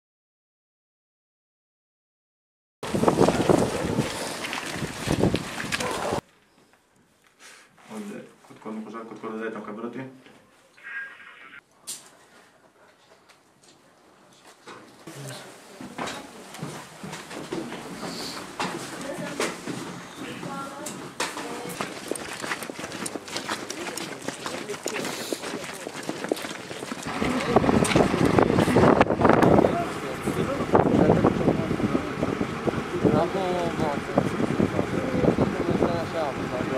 Операция ЦАХАЛа в деревне Кусра
7 декабря 2017 года
Пресс-служба ЦАХАЛа